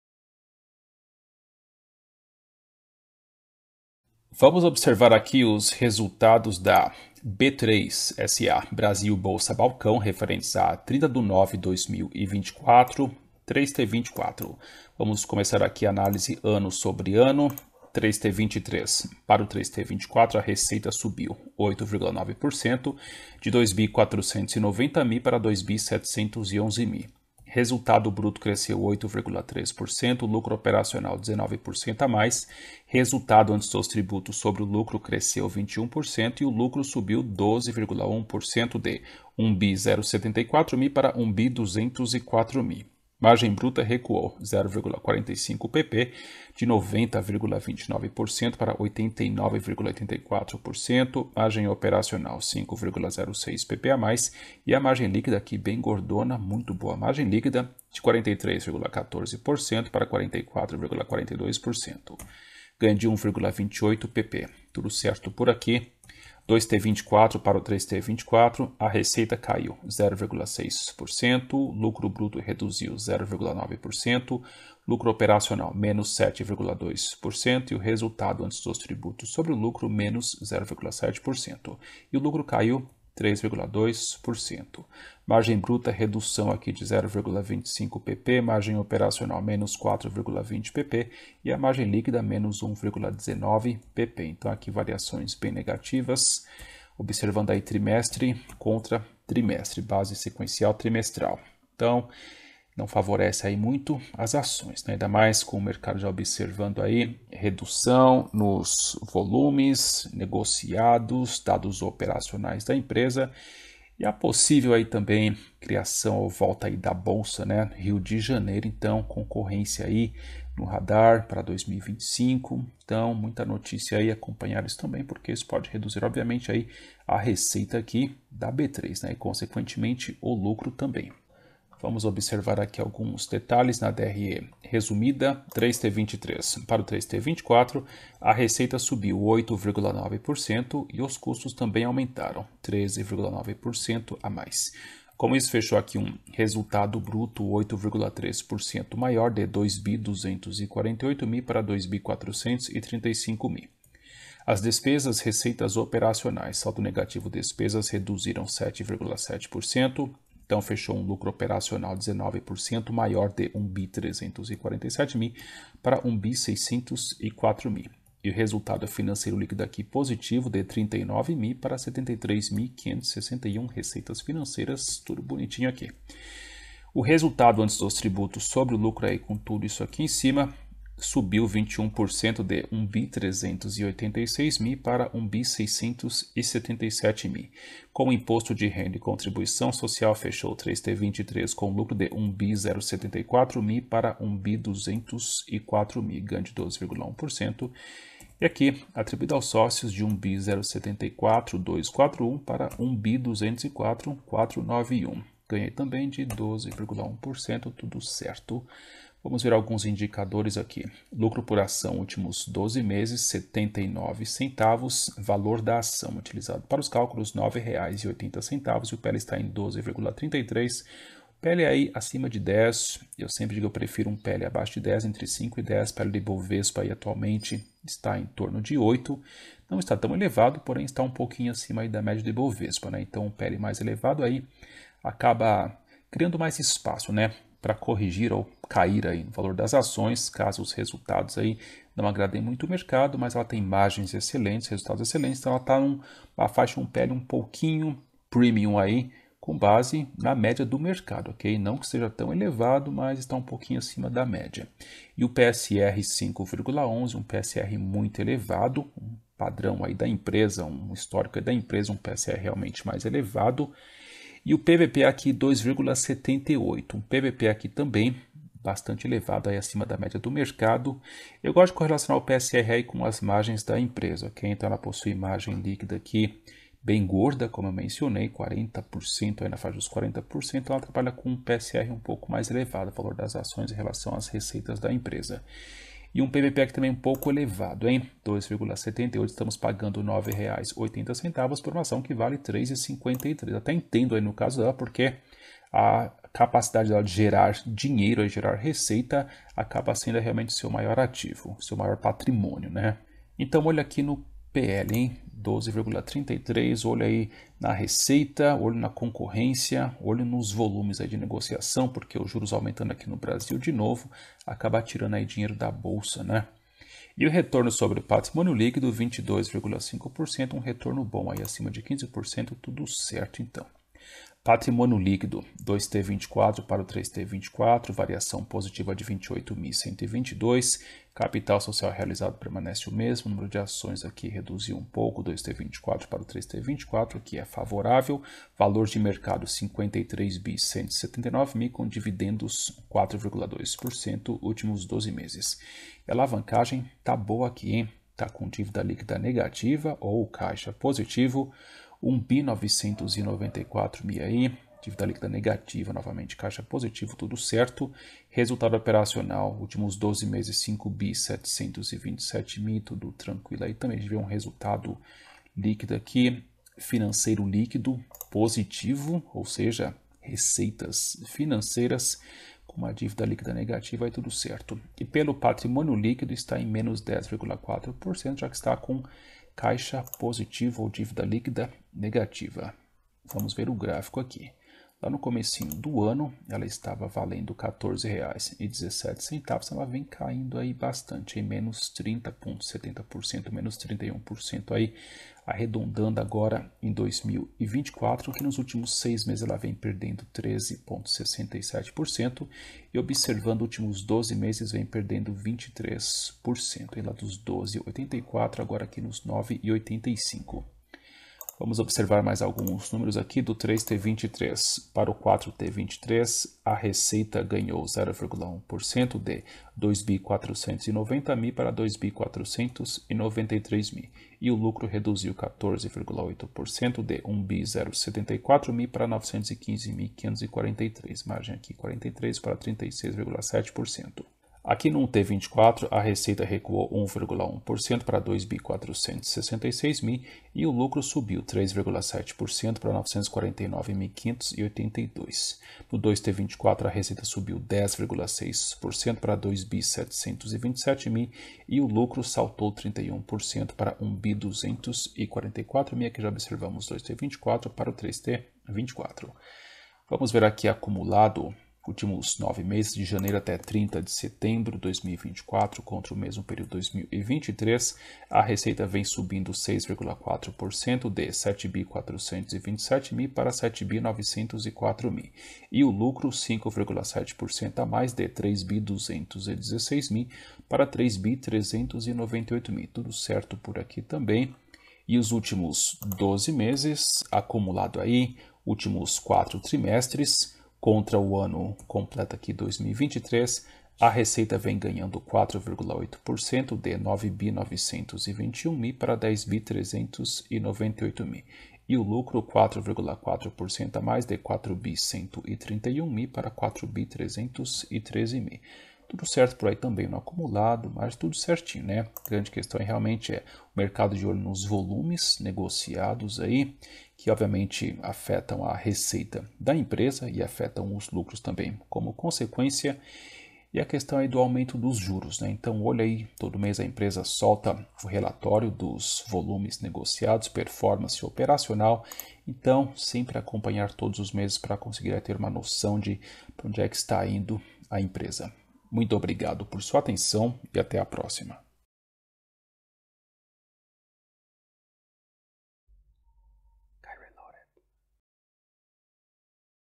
B3: Receita e Lucro Avançam no 3T24 A/A; Ações Caem com Operacional, Fiscal e SELIC Análise B3SA3
12 Dicas Para Não Perder Seu Dinheiro Ao Investir Em Ações